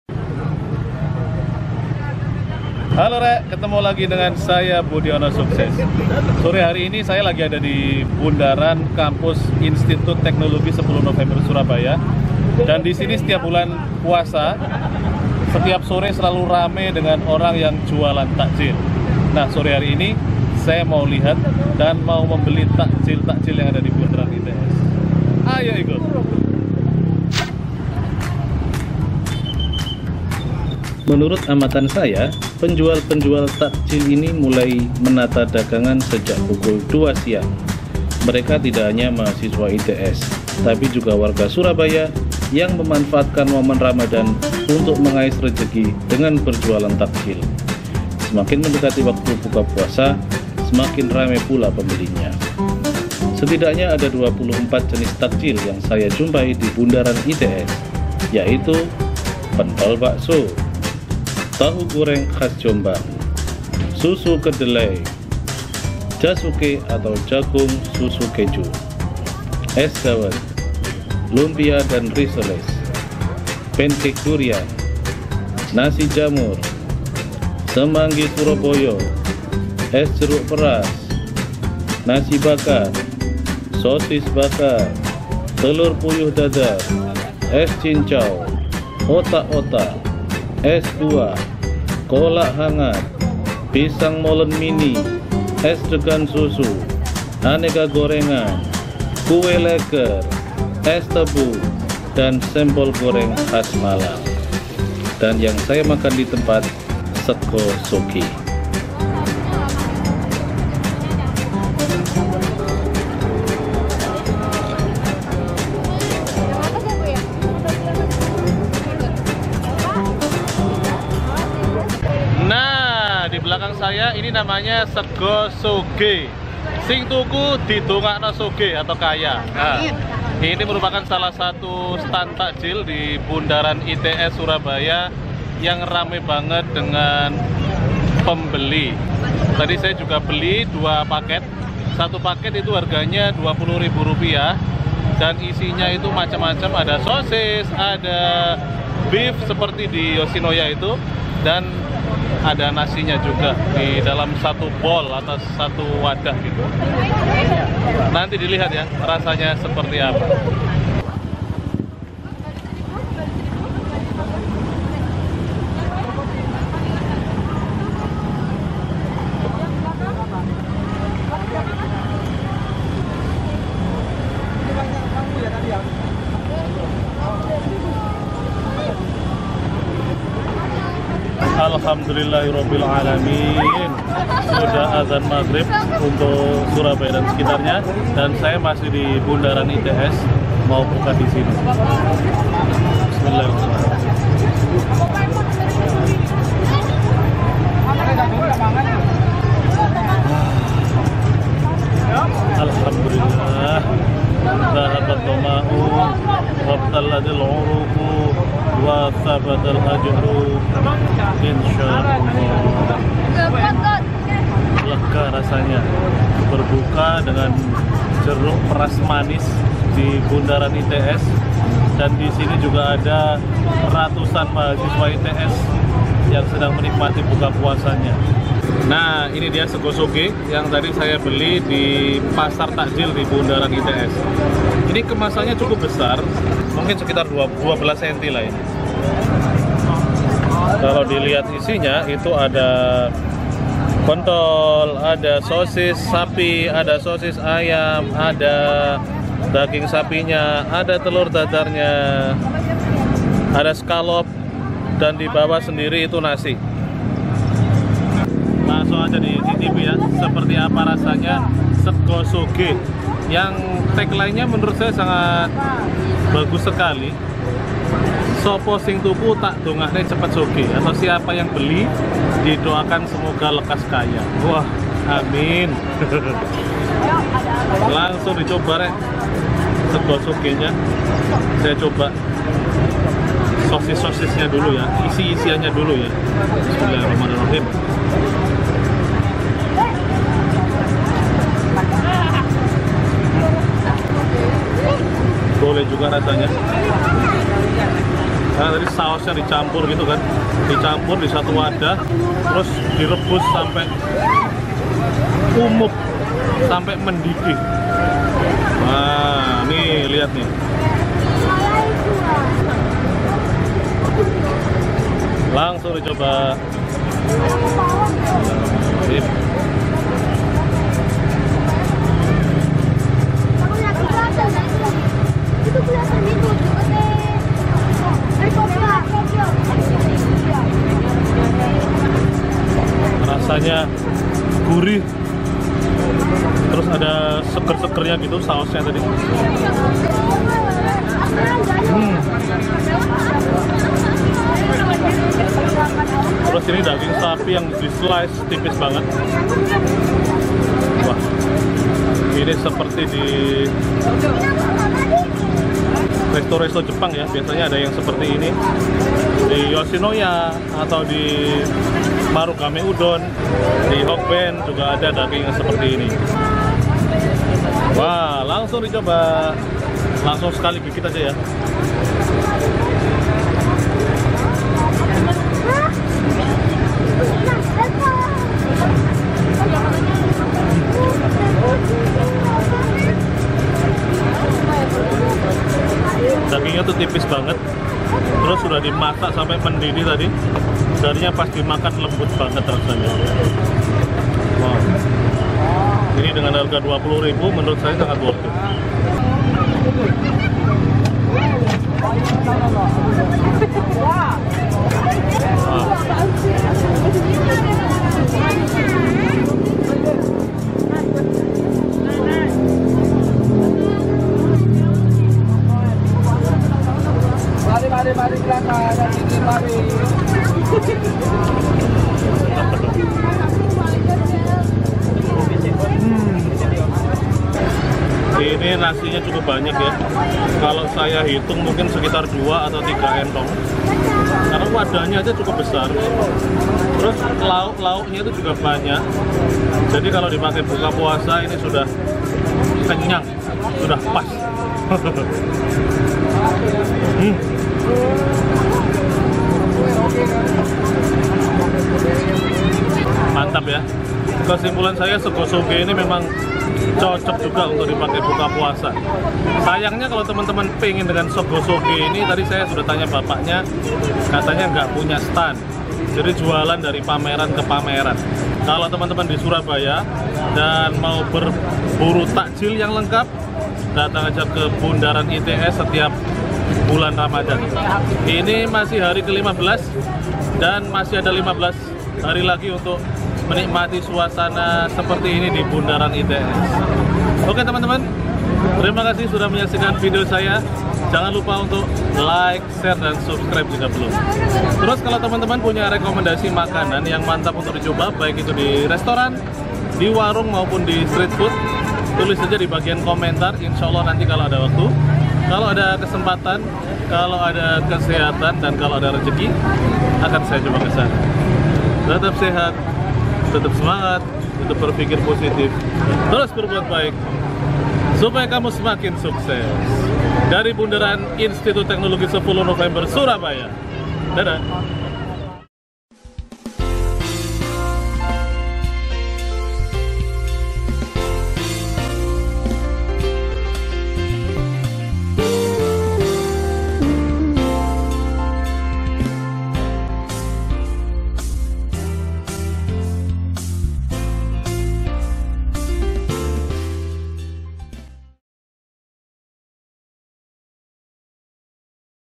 INI DIA 24 TAKJIL BUKA PUASA YANG BISA DIBELI DI BUNDARAN ITS
Taman Bundaran Air Mancur ITS Surabaya setiap Ramadan sore selalu dipenuhi para penjual dan pemburu takjil. Ini dia daftar 24 takjil yang dapat dibeli di Bundaran ITS. Ramadan Market yang selalu berulang setiap bulan puasa

Termasuk di dalamnya SEGOSOGE, Sing Tuku Tak Dungakne Cepet Sogeh. Yang Beli Saya Doakan Cepat Kaya. SEGO SOGE salah satu Pahlawan Ekonomi dari Surabaya